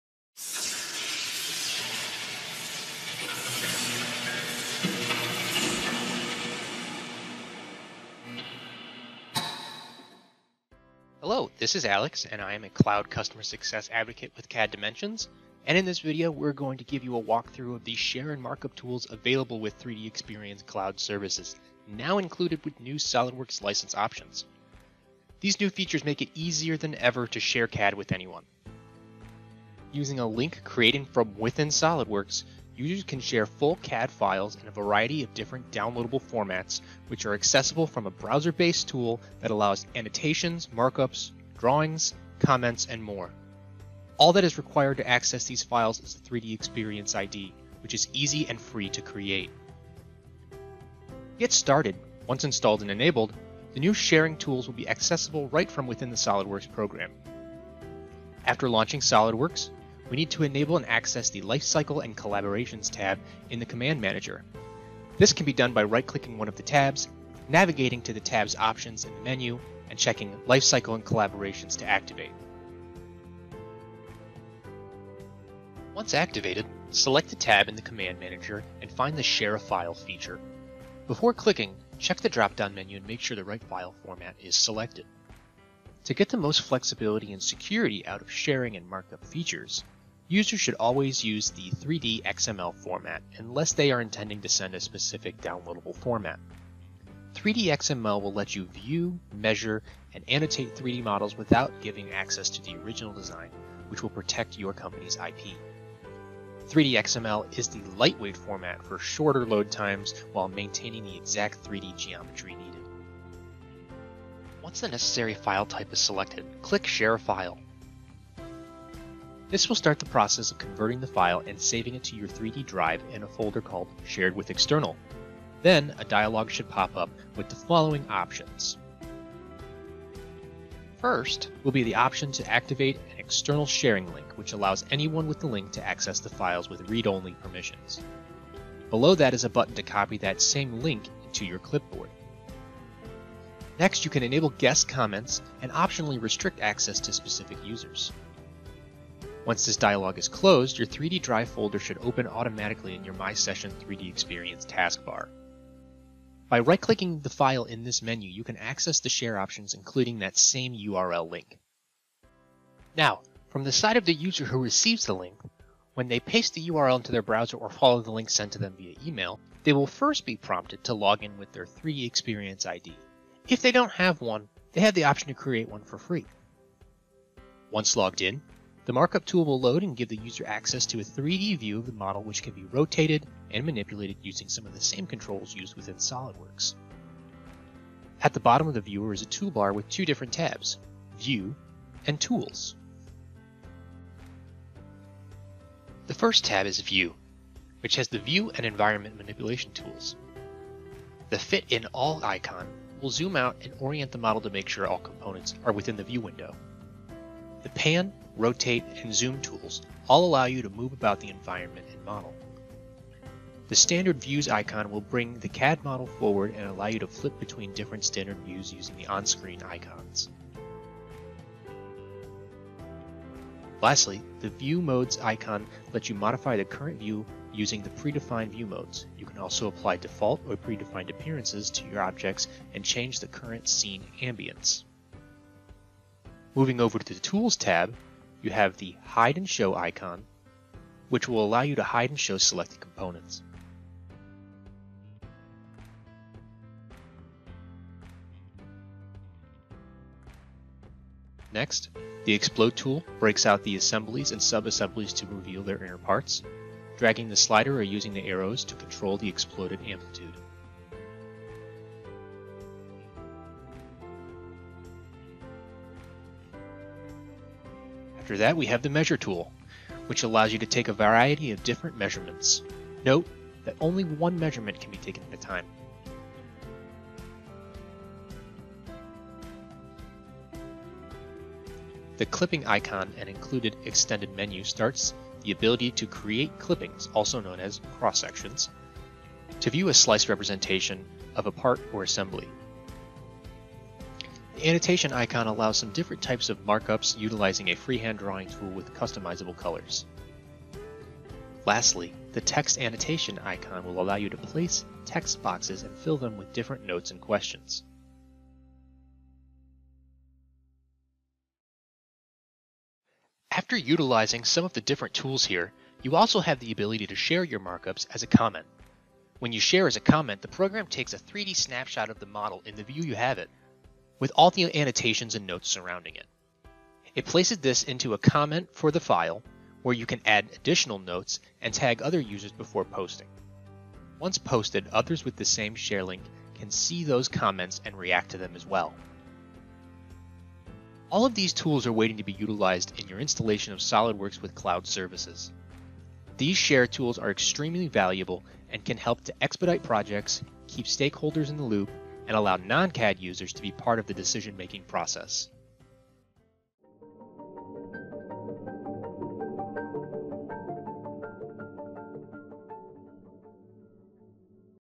Share and Markup Overview in 3DEXPERIENCE
This video provides an overview on the value of the Share and Markup tools as well as step-by-step instructions for getting started.